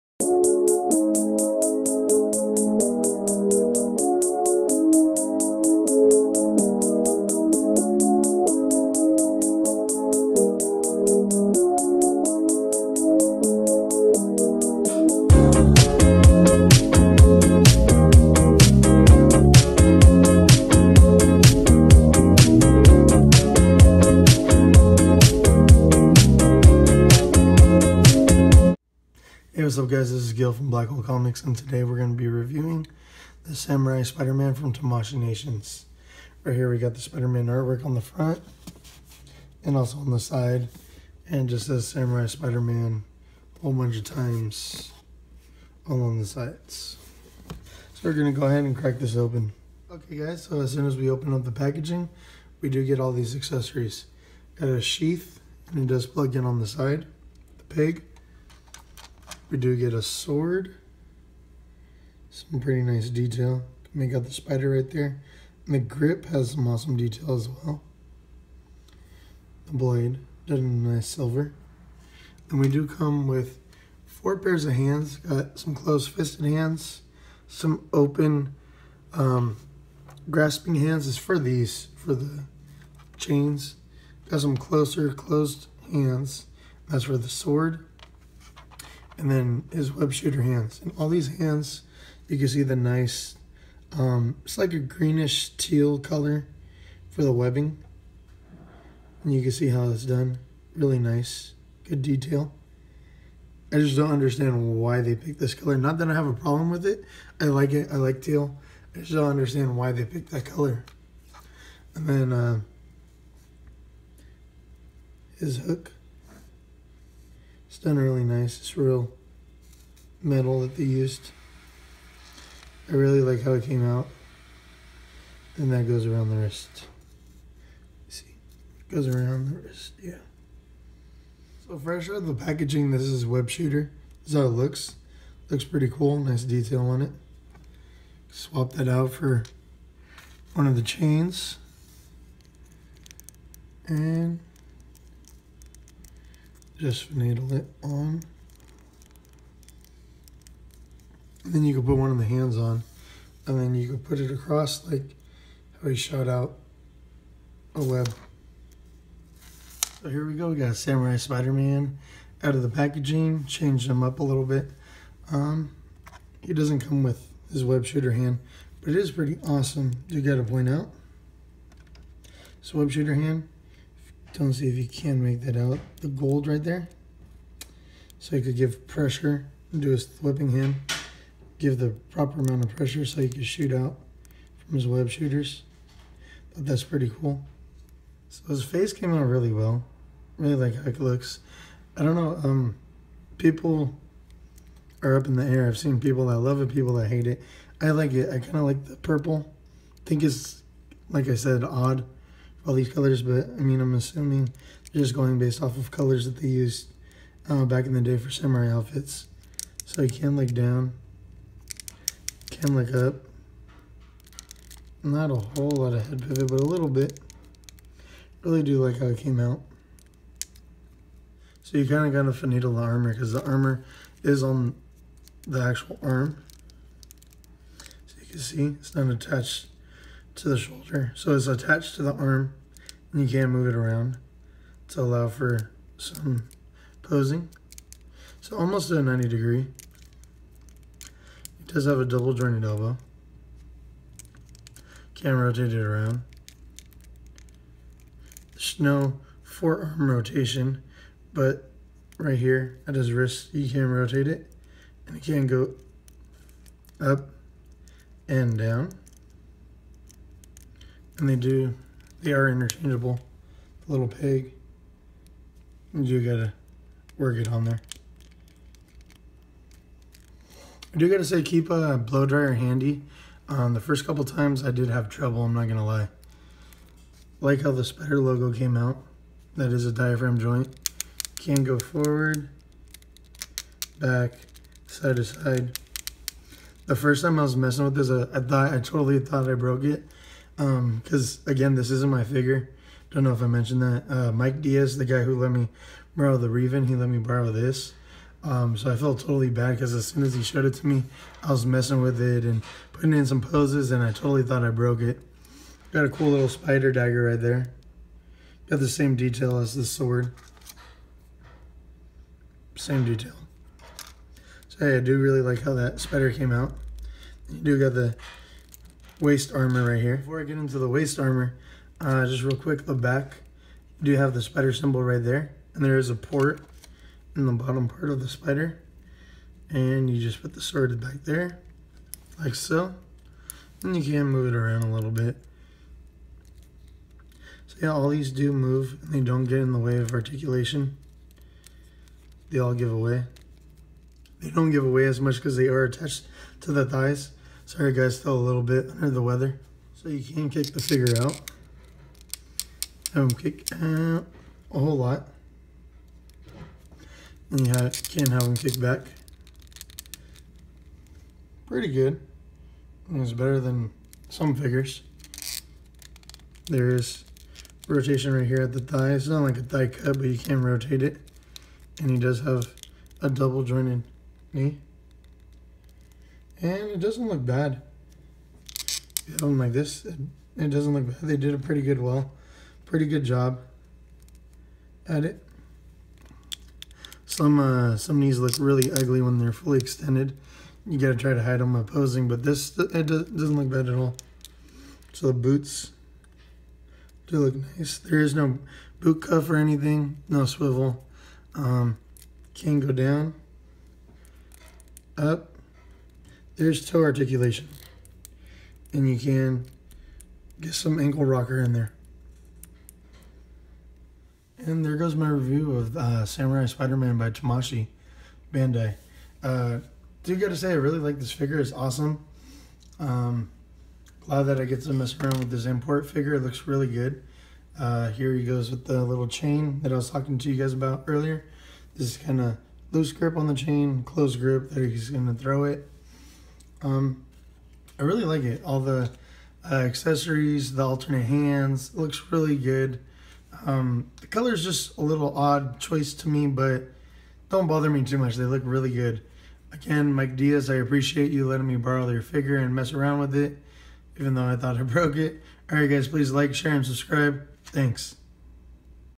Bandai Tamashii nations: Samurai Spiderman
Join me as I review Samurai Spiderman done by Bandai Tamashii Nations. A big thank you to Mike Diaz for letting me review another one of his figures.